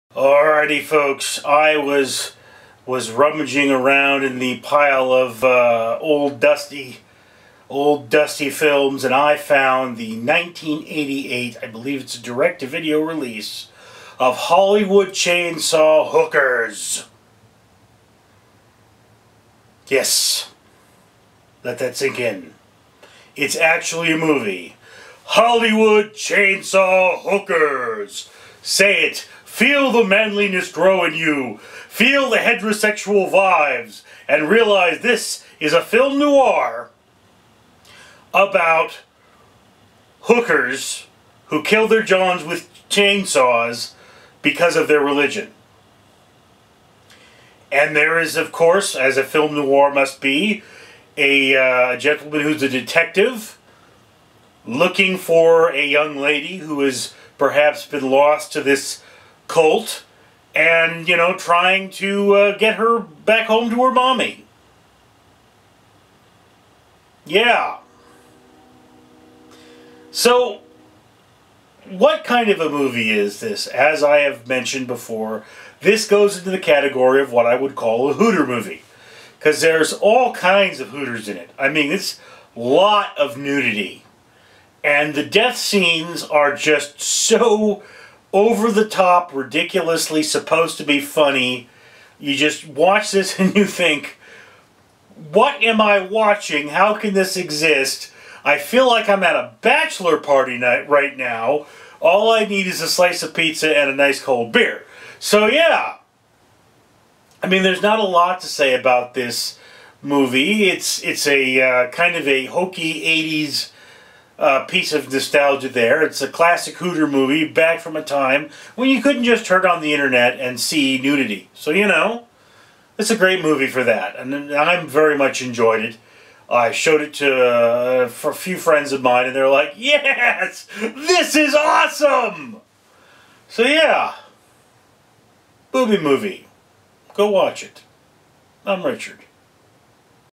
Hollywood Chainsaw Hookers (1988) Review
Hookers in Tinseltown express their religious freedom and kill some dudes.